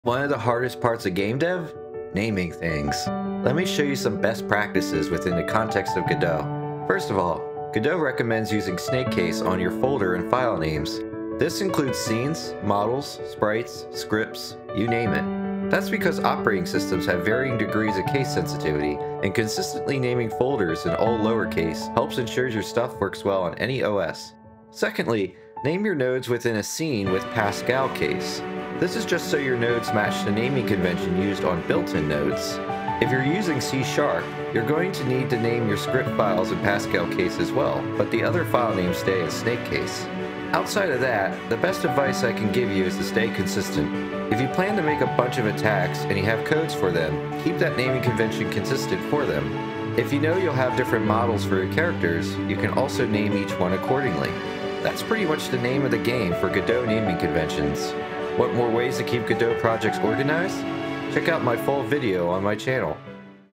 How to Name Stuff in Godot Projects
Here's how you can name things better in your Godot projects!